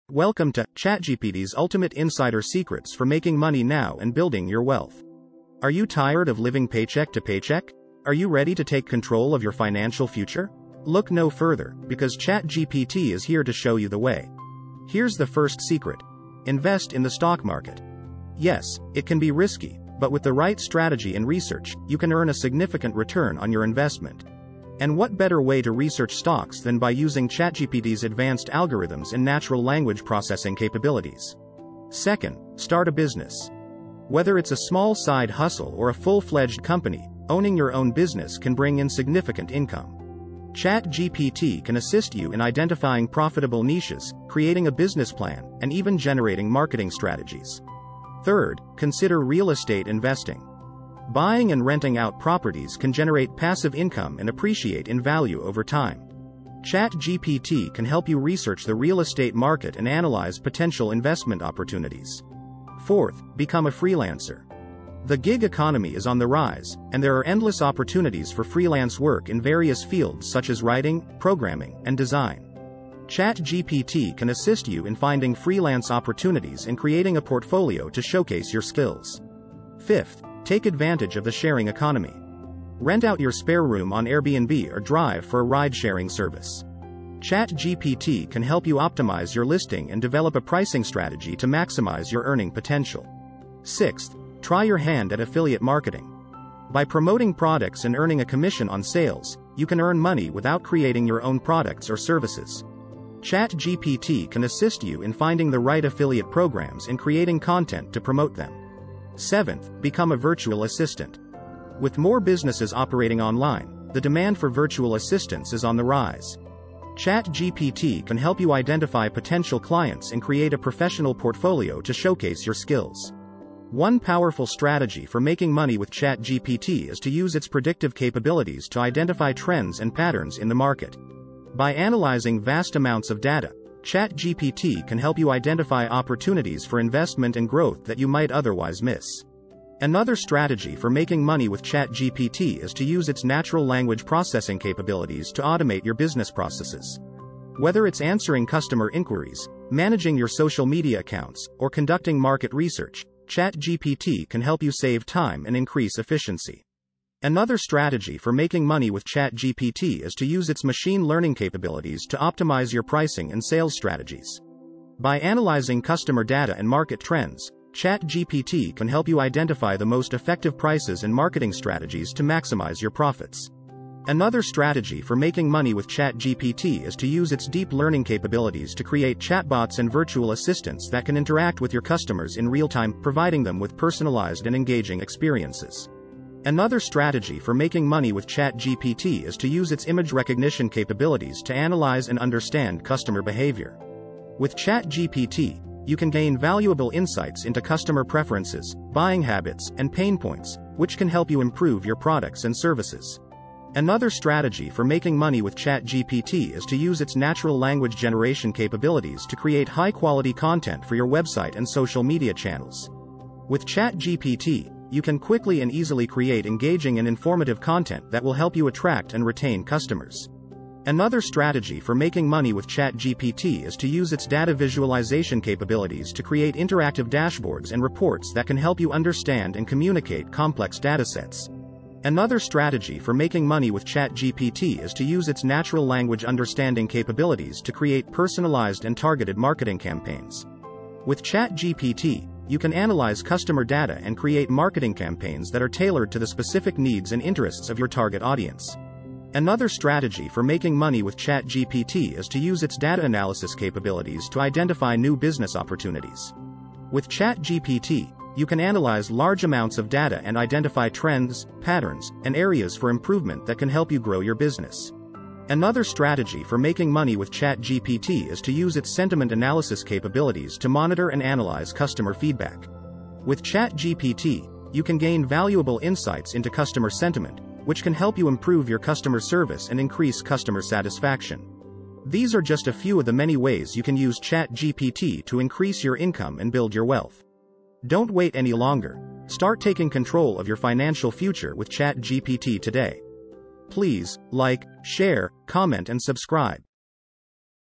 ChatGPT's ULTIMATE Insider Tips for Making Money Now and Building Your Wealth
Welcome to our video on ChatGPT's ULTIMATE Insider Strategies for Making Money Now and Building Your Wealth. In this video, we will be sharing with you exclusive and unique ways to make money using the power of ChatGPT. From innovative investment opportunities to new ways of monetizing your skills and knowledge, we've got you covered. We know that making money can be tough and that's why we've developed these strategies specifically to help you succeed. Whether you're looking to make a quick buck or want to build long-term wealth, we've got the tools to help you get there. So sit back, relax, and get ready to learn how to make money now with the help of ChatGPT.